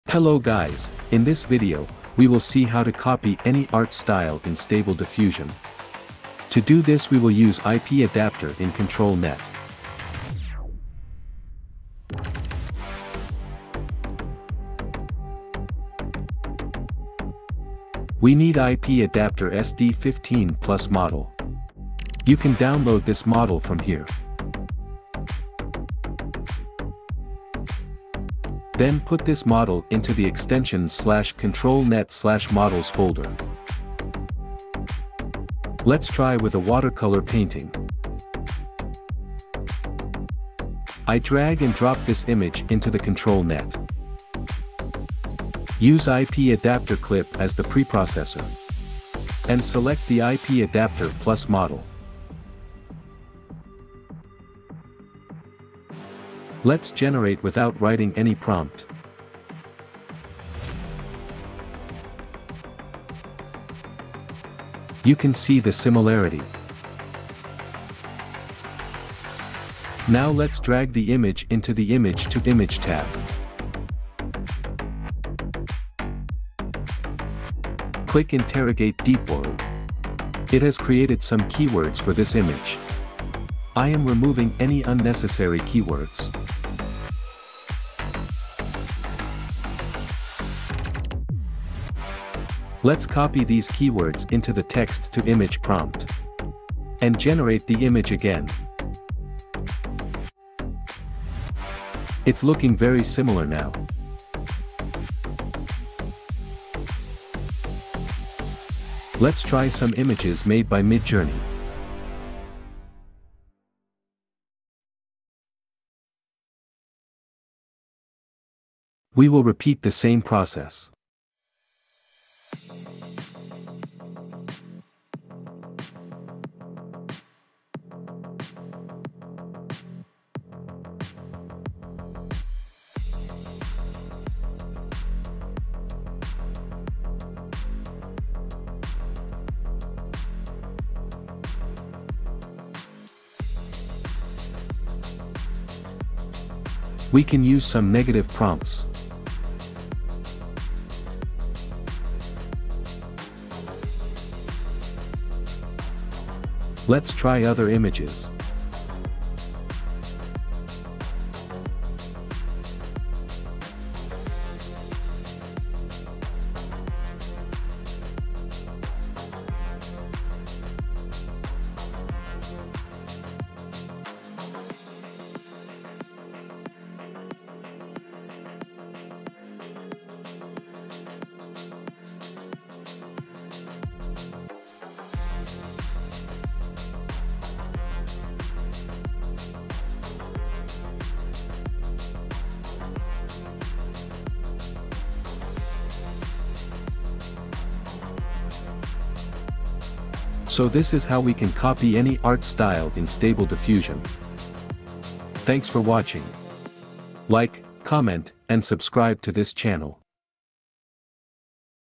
Copy Any Art Style in Stable Diffusion
In this video, we will see how to copy any art style in Stable Diffusion.